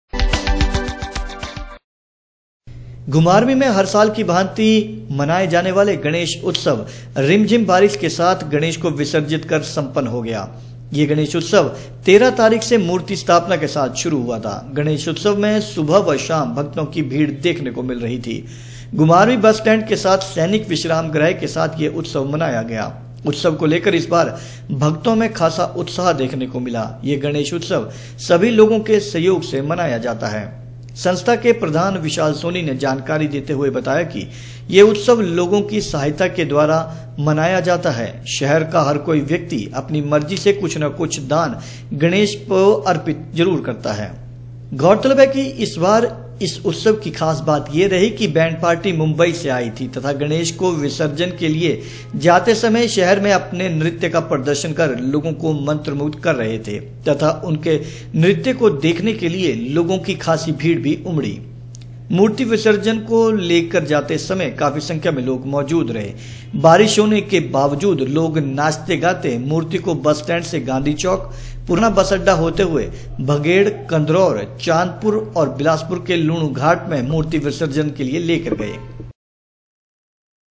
घुमारवी मे उत्सव रिमझिम बारिश के साथ गणेश को विसर्जित कर संपन हो गया
घुमारवी मे हर साल की भांति मनाया जाने वाला गणेश उत्सव रिमझिम बारिश के साथ गणेश को विसर्जित कर संपन हो गया है। यह गणेश उत्सव 13 तारीख से  मूर्ति की स्थापना के साथ शुरू हुआ था। ।गणेश उत्सव मे सुबह व शाम भक्तों की काफी भीड़  रही हैं ।